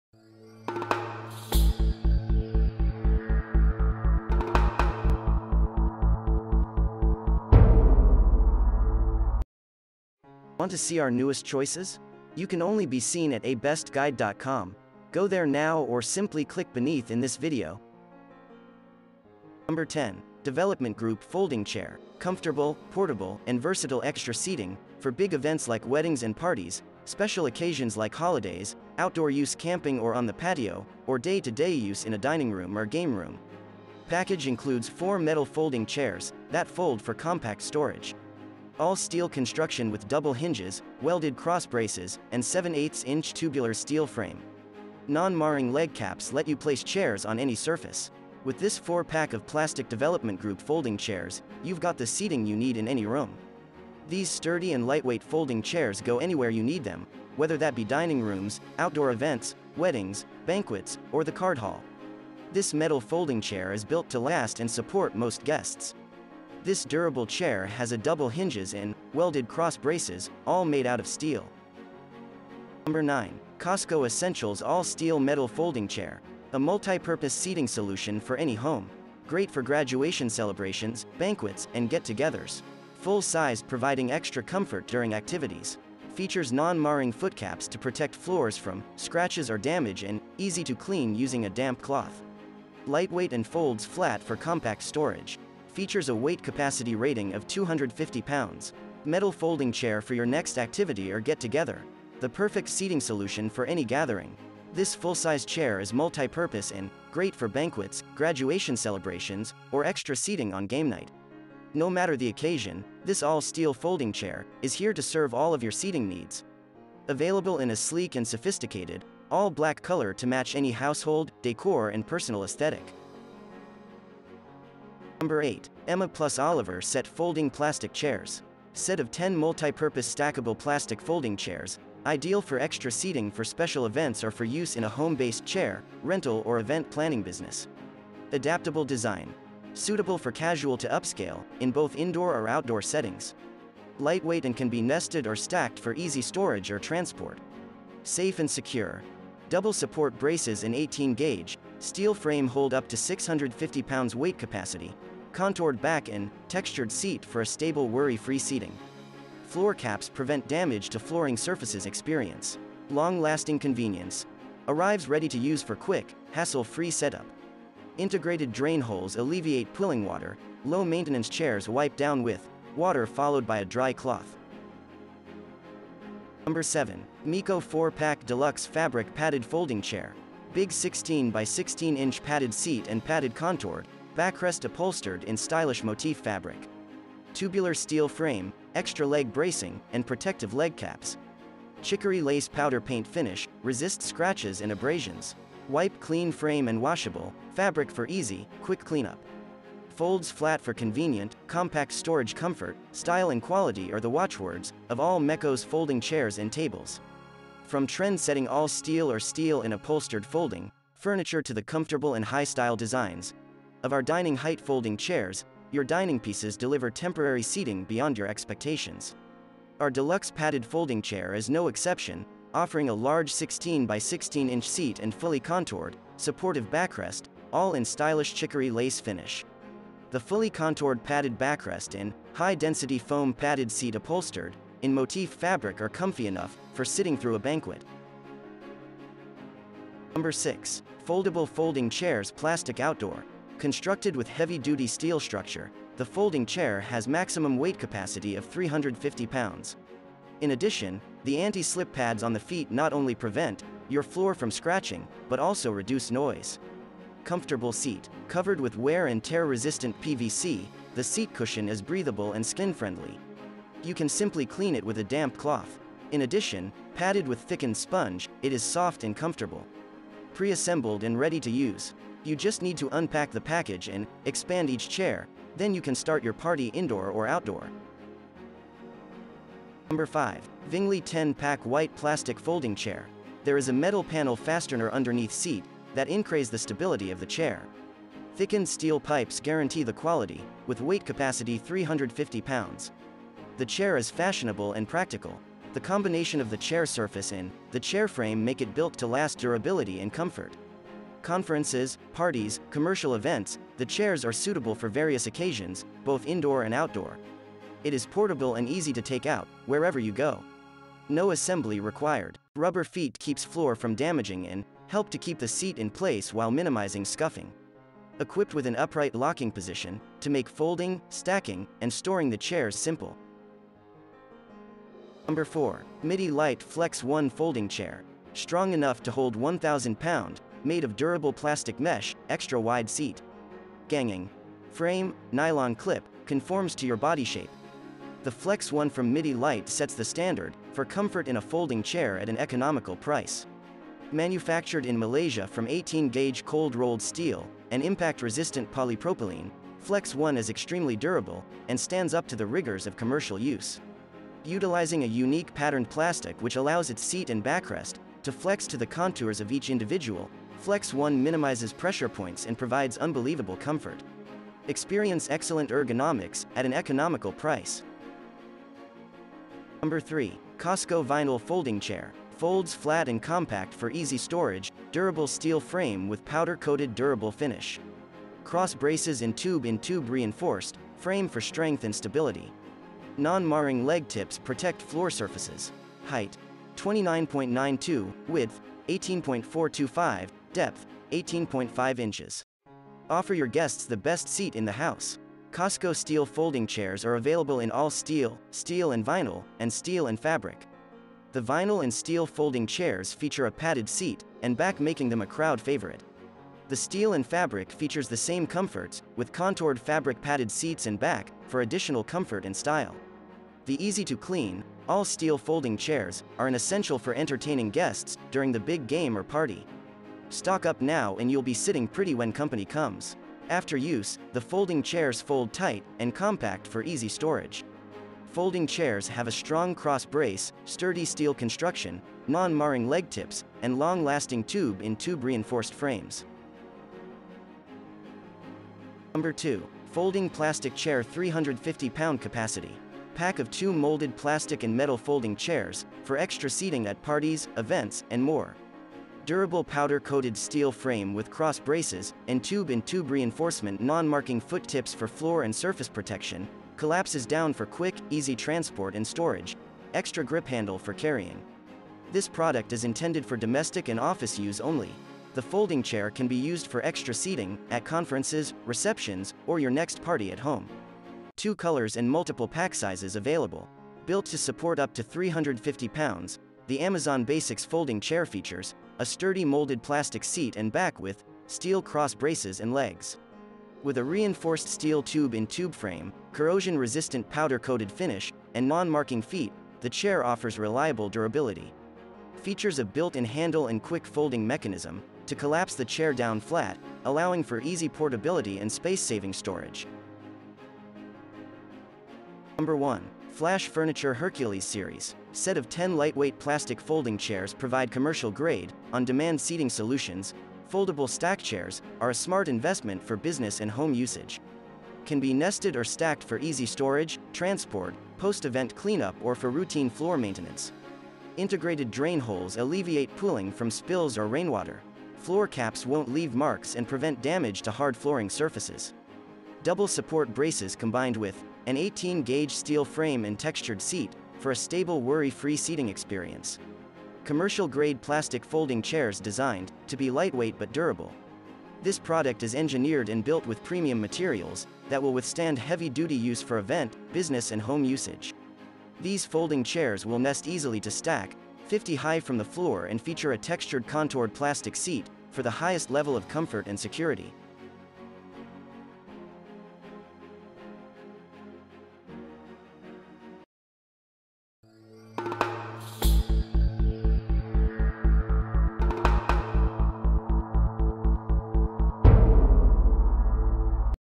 Folding Chairs - Best Folding Chairs in 2022 [Top10]
The Best Folding Chairs are portable and can be folded flat to a smaller size when not in use, making them very easy to carry and store. Folding chairs are specifically made for outdoor leisurely use like camping, hiking, or outdoor picnic.

Here Are The Top 10 Best Folding Chairs